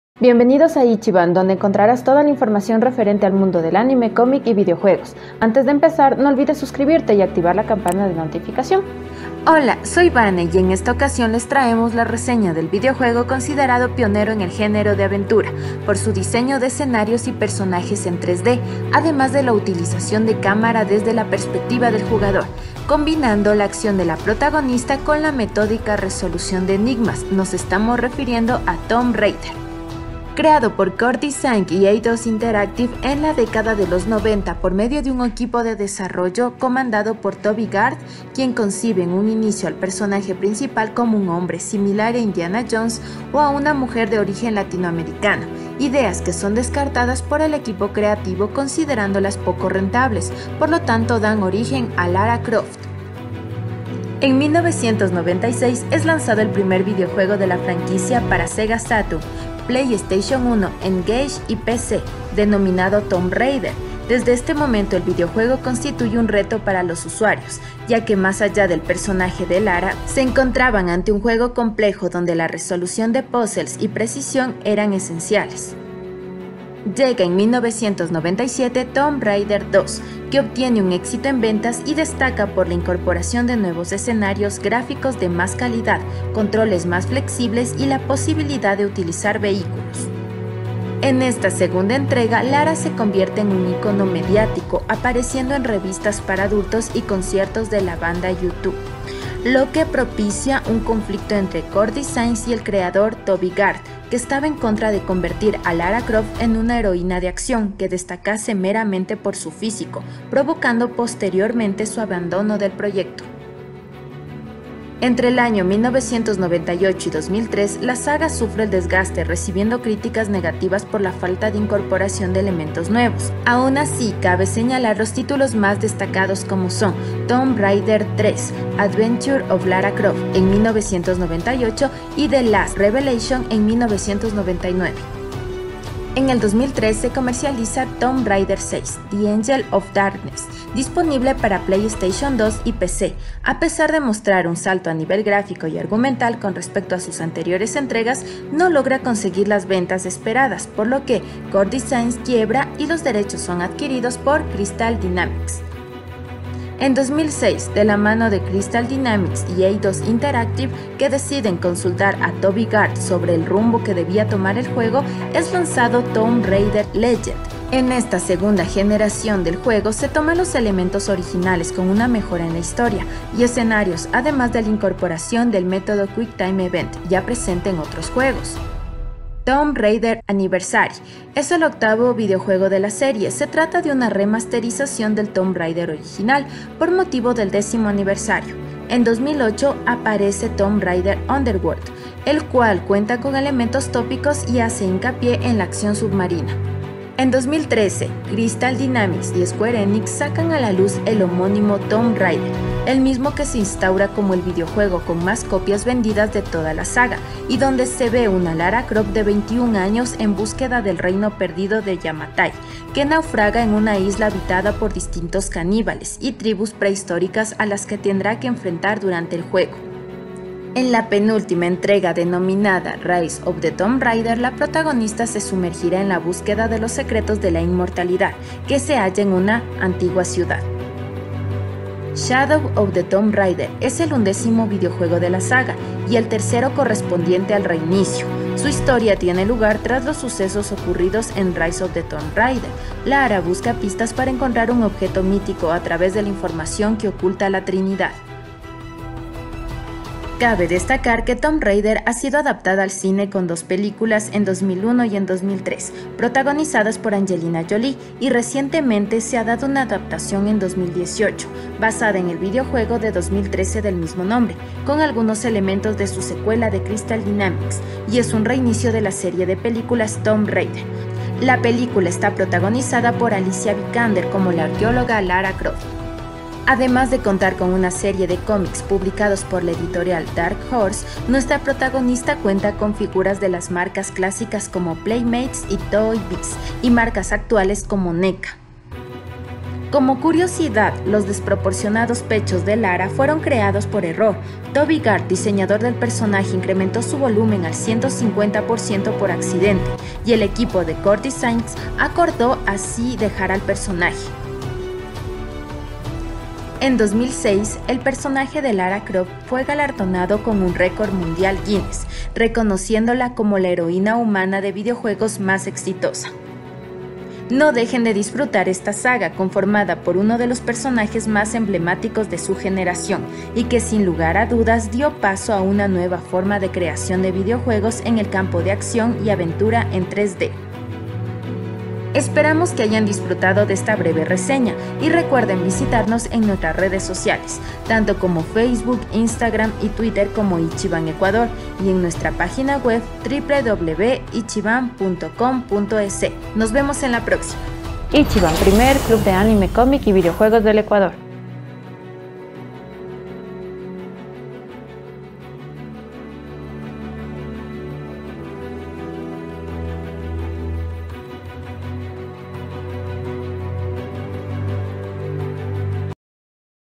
Reseña Tomb Raider - Ichiban
Una breve reseña gamer sobre la gran saga de videojuegos Tomb Raider... Que lo disfruten!

Créditos: Vanessa Navas, Oswaldo Araujo, 3V Advisor.